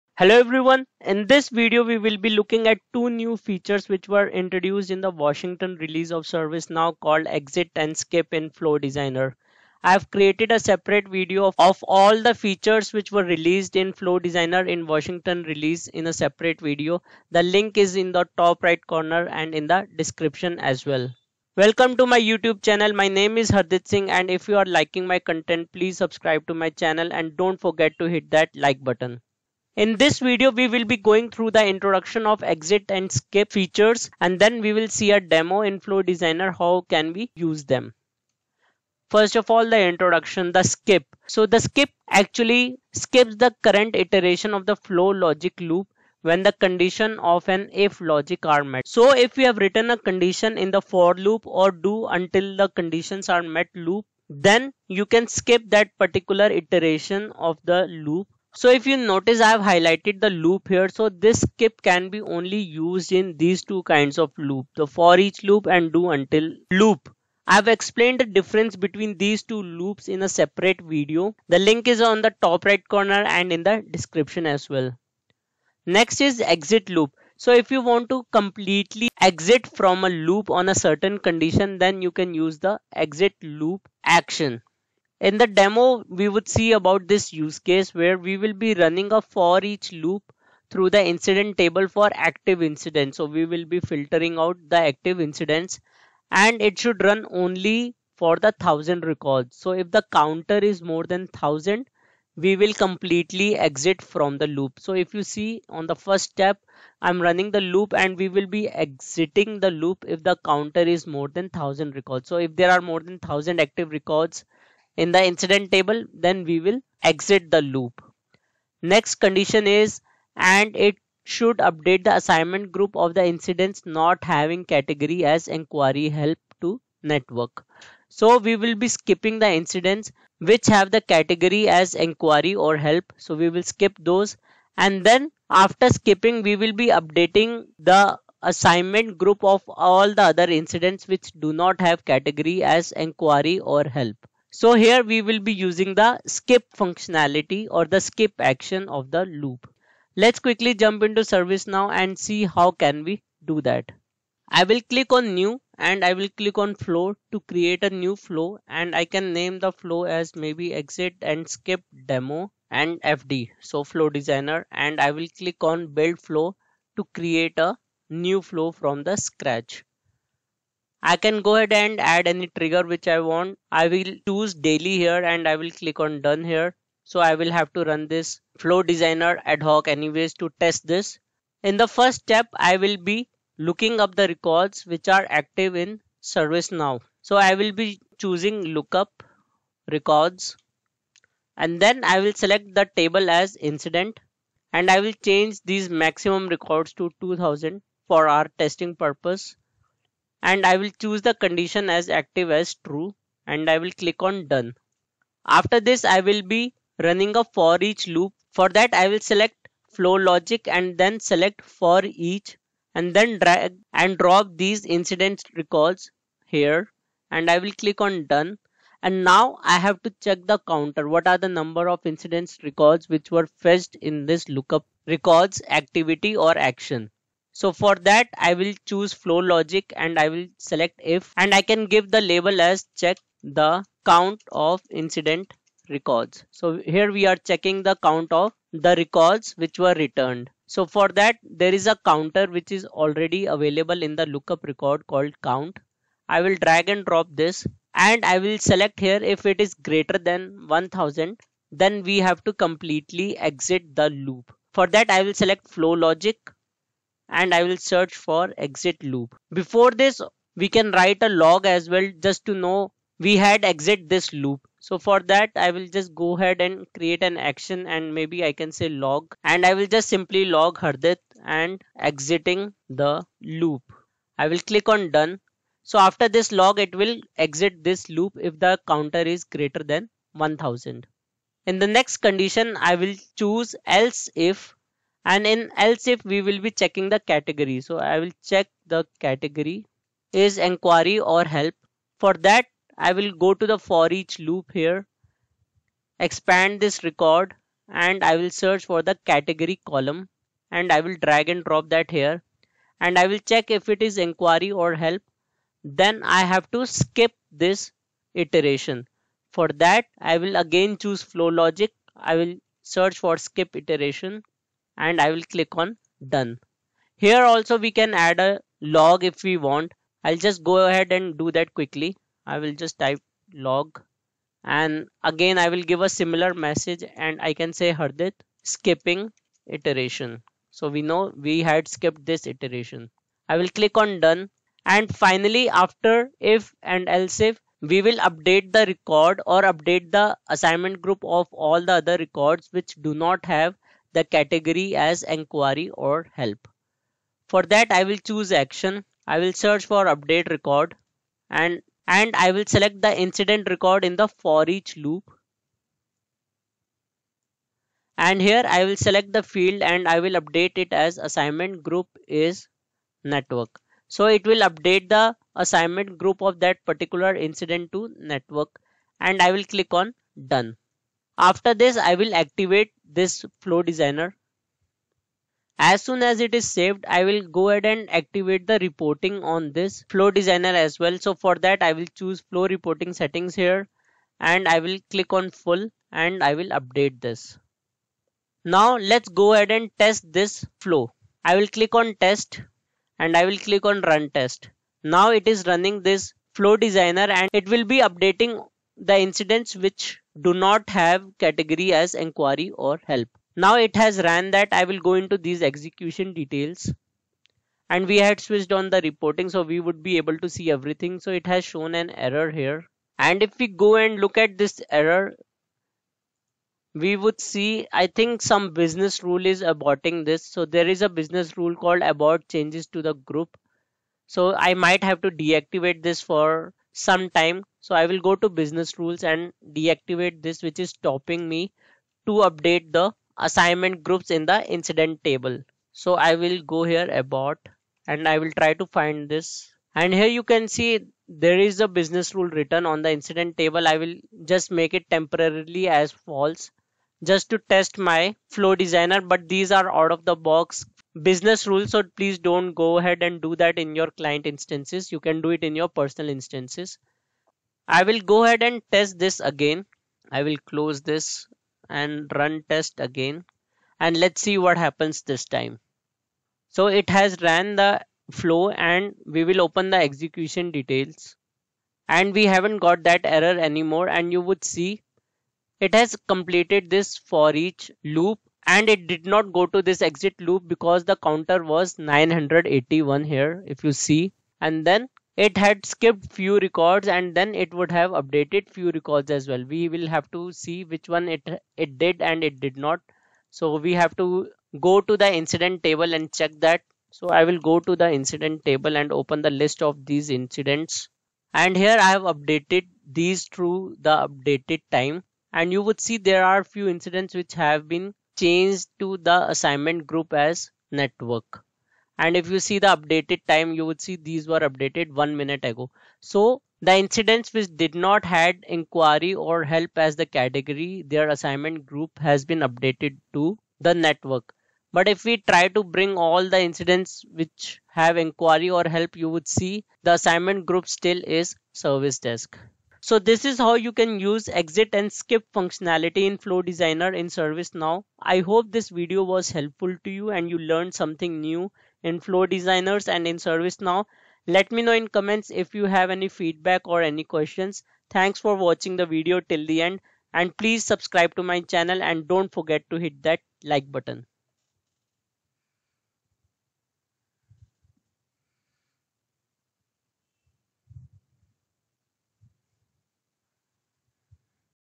Exit and Skip Loop in Flow Designer in ServiceNow
Skip the current iteration of a flow logic loop when the conditions of an If flow logic are met.
Exit loop is used for completely exiting  a loop on a certain condition.

00:00 - Introduction
01:30 - Benefits of Reference fields
02:40 - Demo